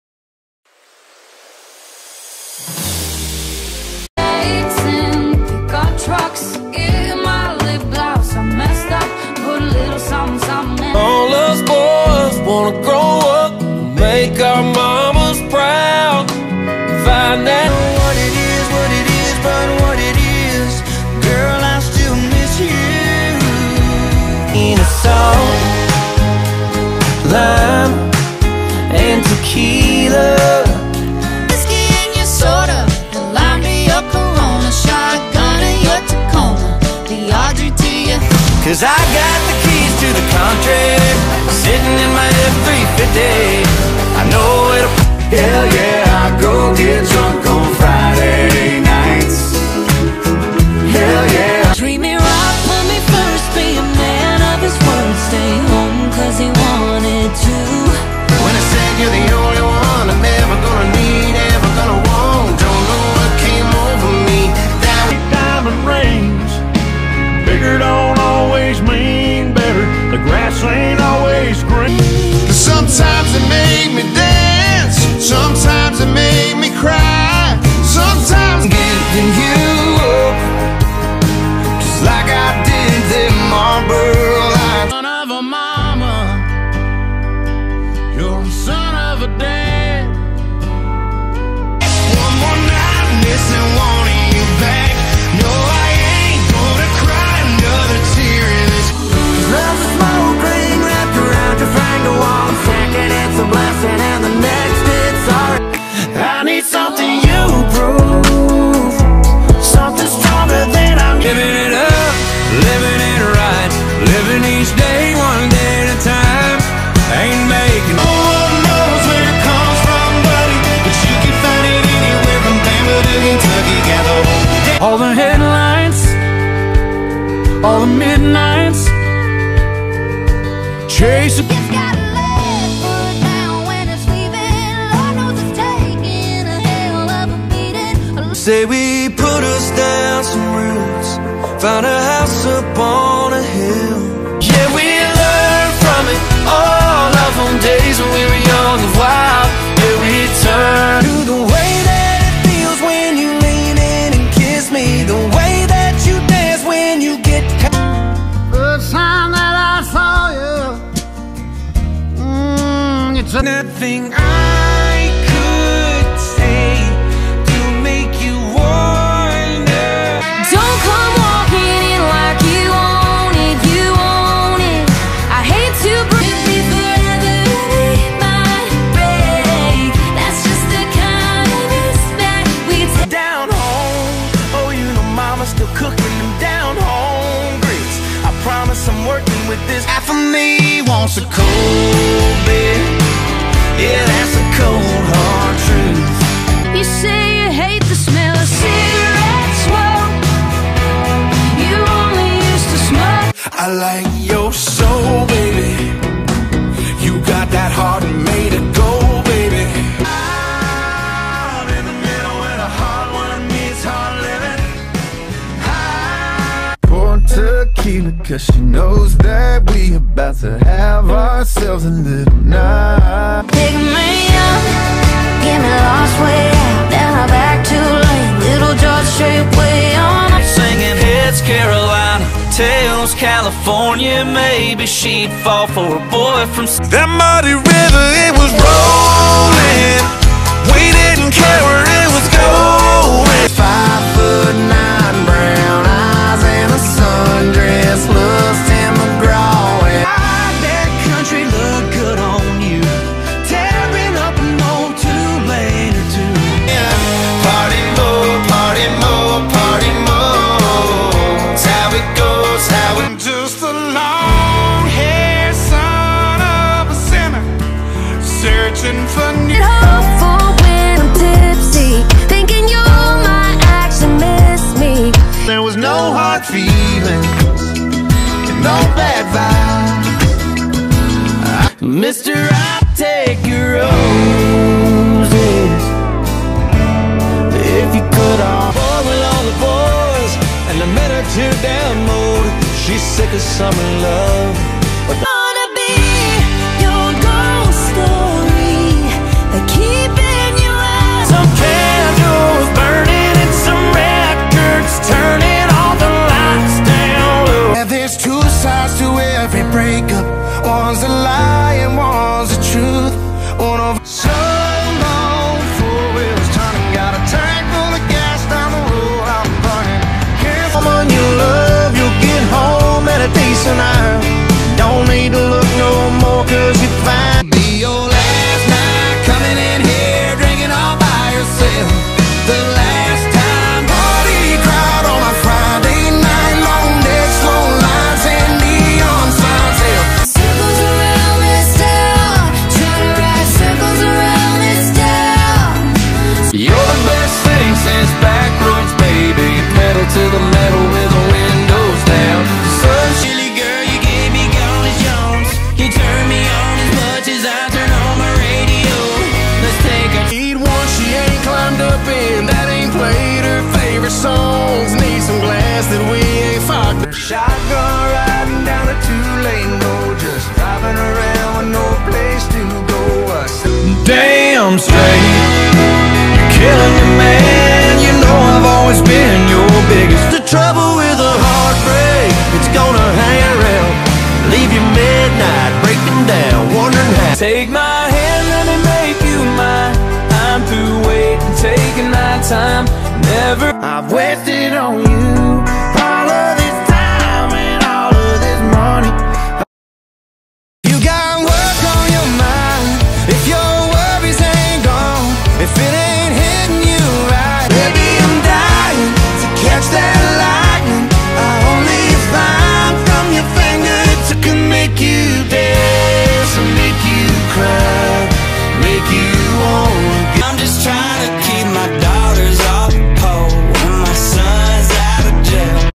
Billboard Top 60 Country Airplay Chart (July 2, 2022)
Billboard Country Airplay: Top 60 songs played on radio stations in a week